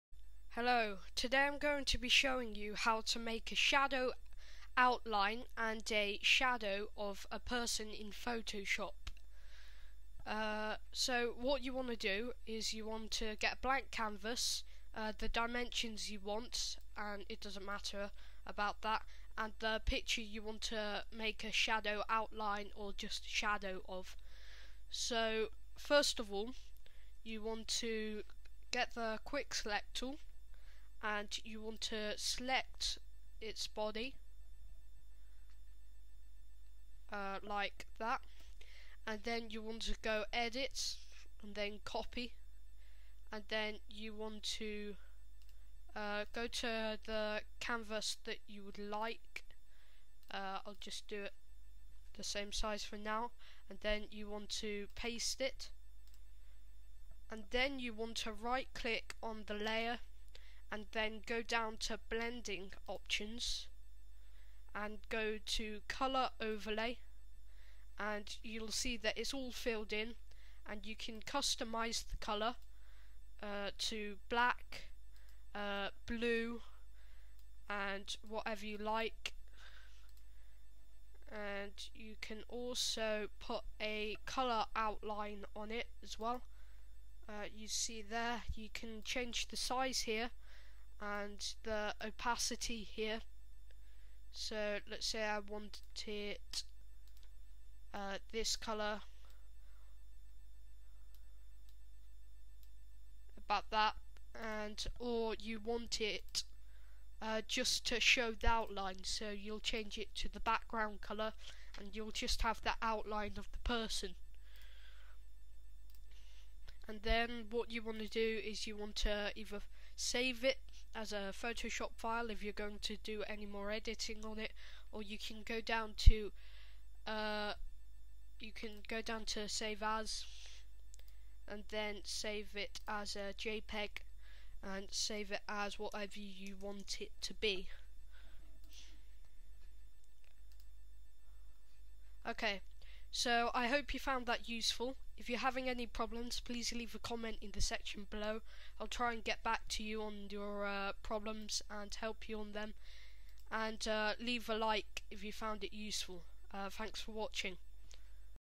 How to make a person into a shadow in photoshop
In this tutorial Artic gadgets demonstrates how to use layer styles to create a cool shadow effect for your photos.